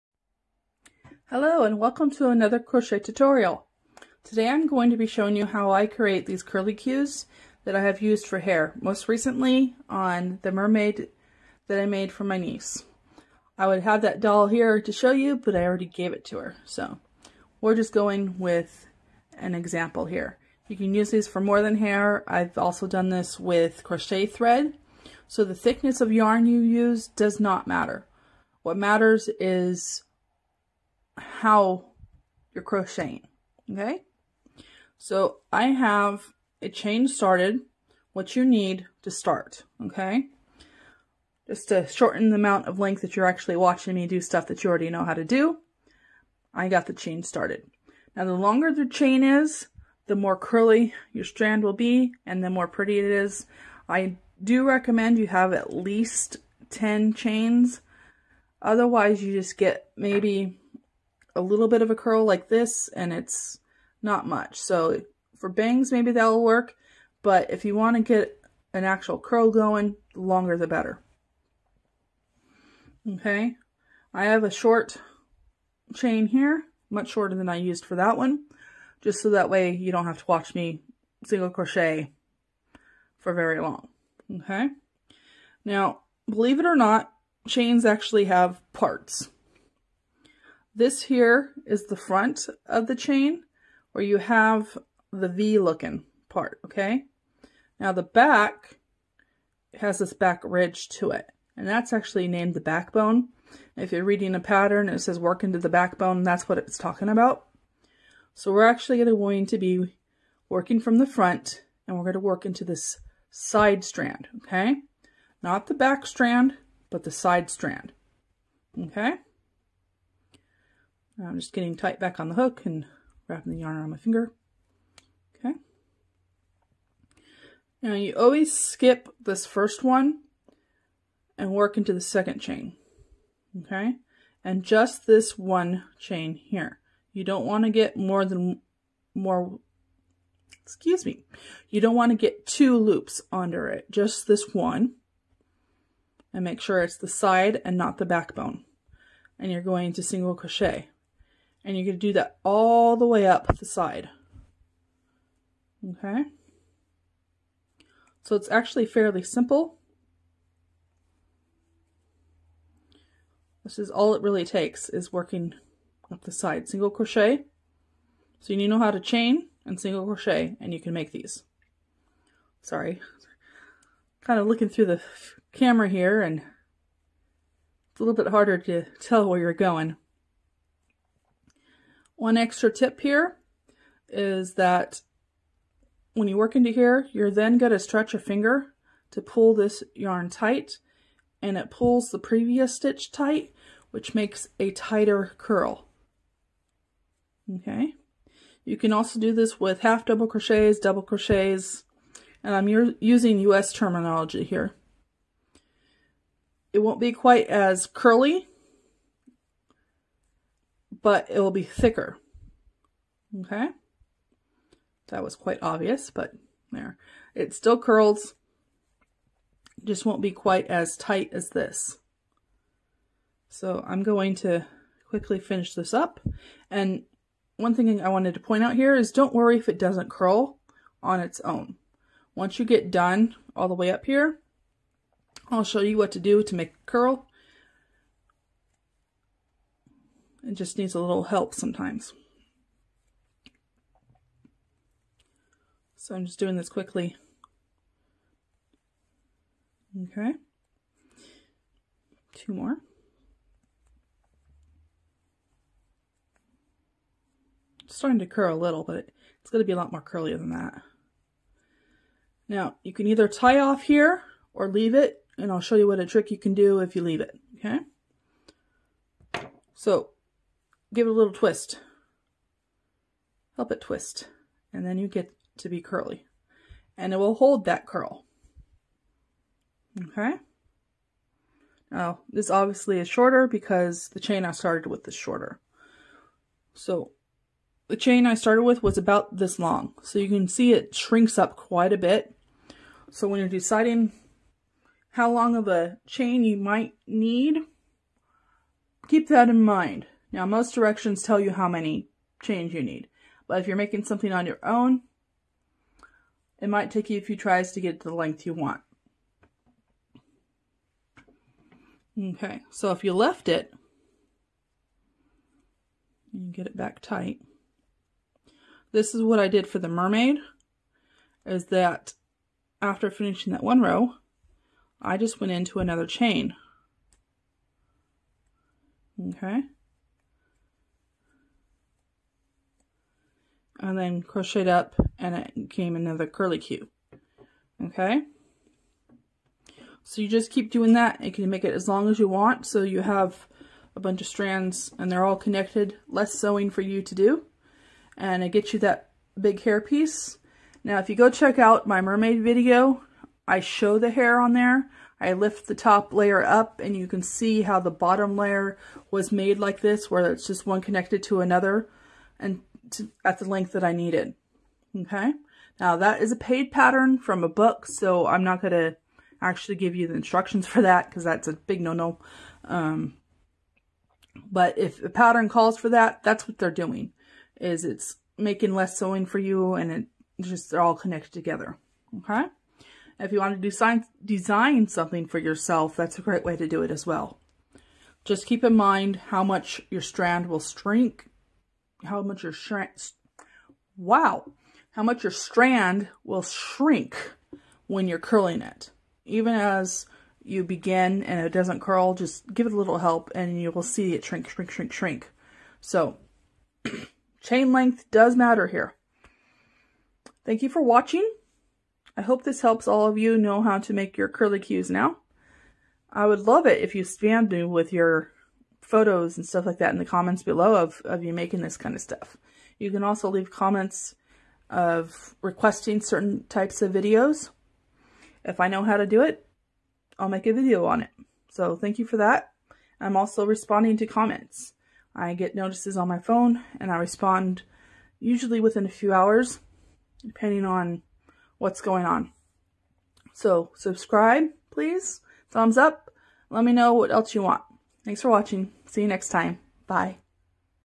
How To Crochet Curly-Q Hair
How to crochet curly hair for your Amigurumi creations.  This is how I made the hair for both the mermaid and unicorn I made for my nieces.